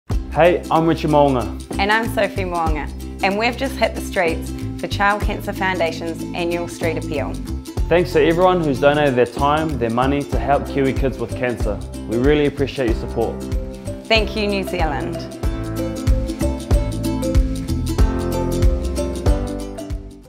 2020 Street Appeal - Richie and Sophie Mo'unga Thank You
Thanks to everyone who collected and donated in our 2020 Street Appeal!